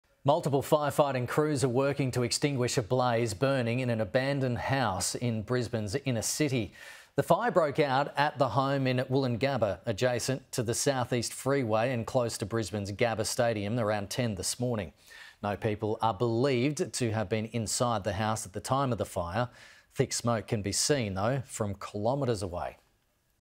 Blaze erupts in abandoned Brisbane home near Gabba stadium
Multiple firefighting crews are working to extinguish a blaze burning in an abandoned house in Brisbane's inner city.

The fire broke out at the home in Woolloongabba adjacent to the South East Freeway and close to Brisbane's Gabba stadium around 10am on Wednesday morning.

Thick smoke can be seen from kilometres away.